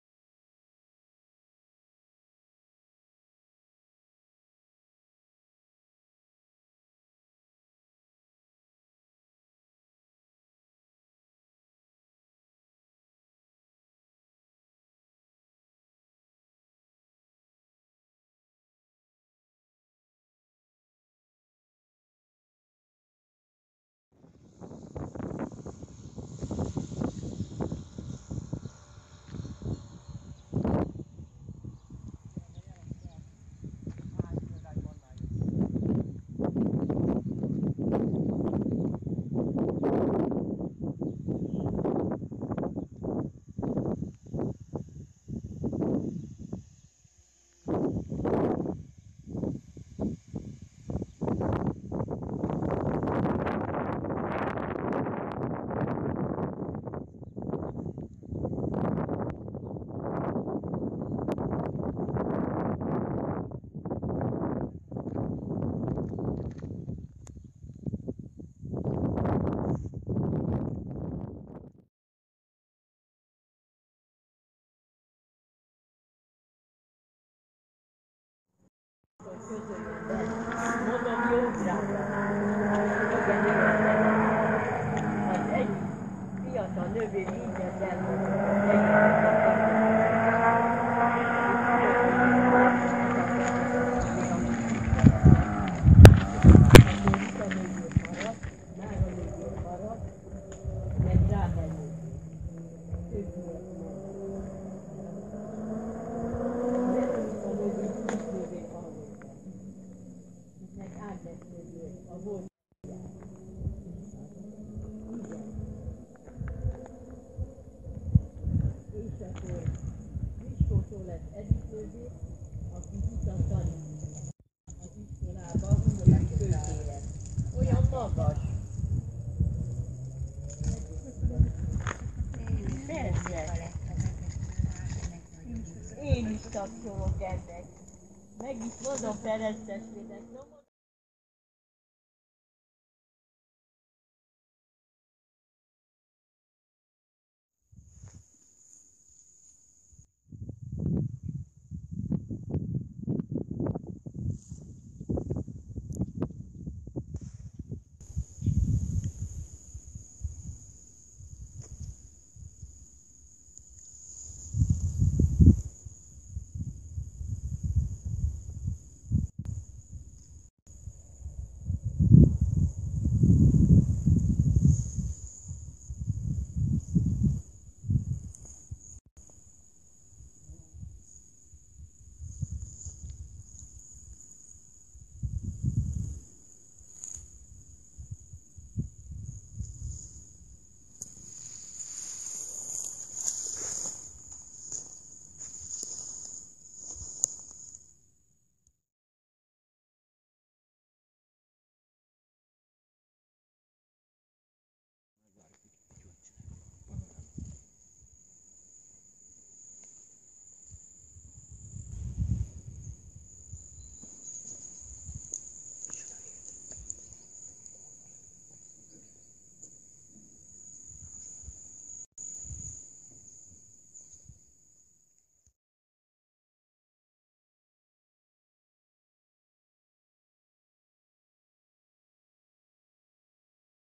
Bánya-Bükk túra - 2015.08.01
Bükkszentkereszt - Fehérkő-lápa - Fehér-kő - Bükkszentlászló - Bükkszentkereszt.